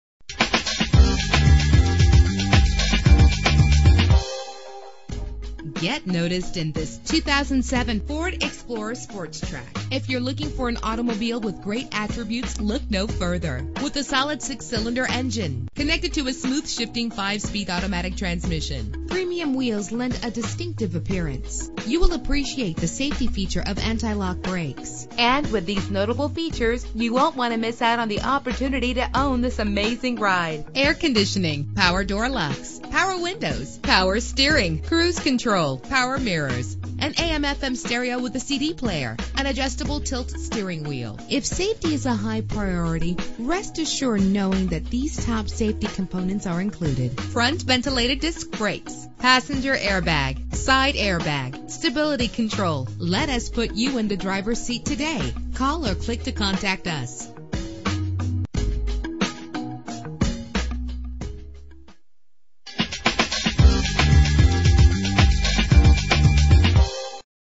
2007 Ford Explorer Sport Trac - Miami FL
Year: 2007
Make: Ford
Model: Explorer Sport Trac
Trim: XLT
Engine: 4.0 liter 6 cylinder 12 valve
Transmission: 5-Speed Automatic
Color: Red Fire Clearcoat
Mileage: 49999
Address:
7339 SW 45th ST